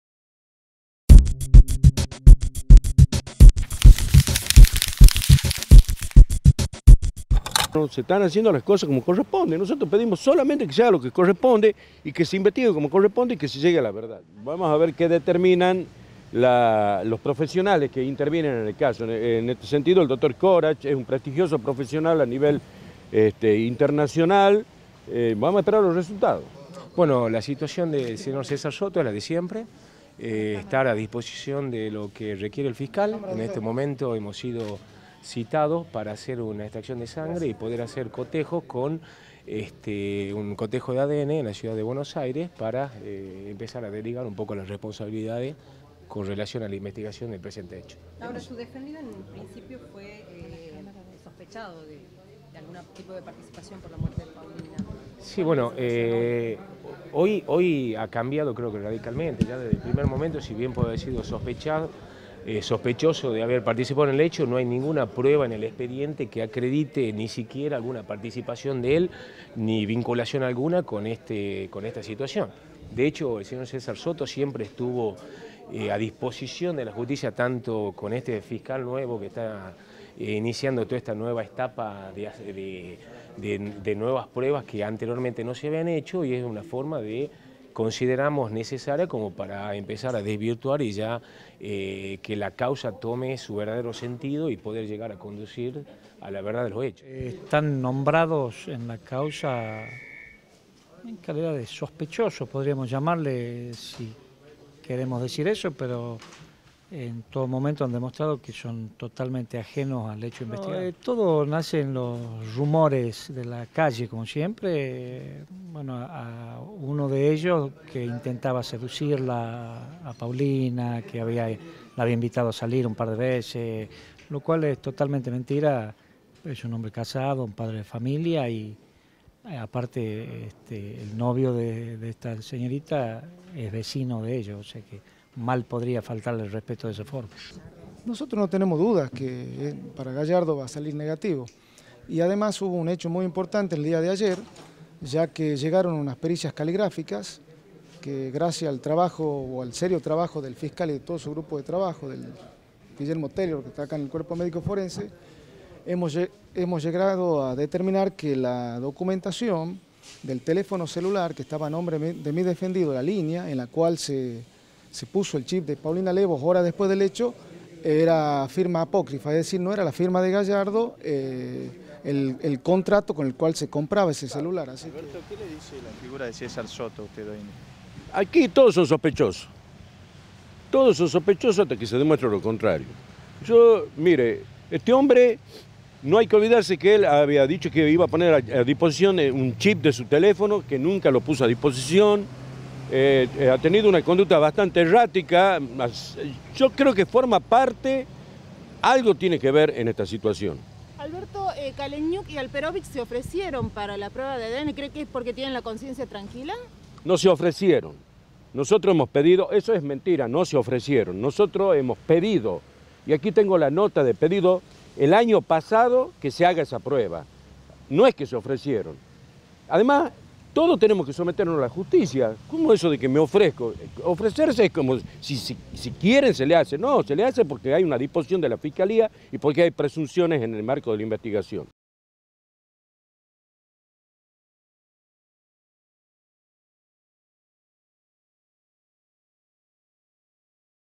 Tomaron muestras de ADN en el caso Lebbos
Alberto Lebbos respaldó la investigación del fiscal López Ávila.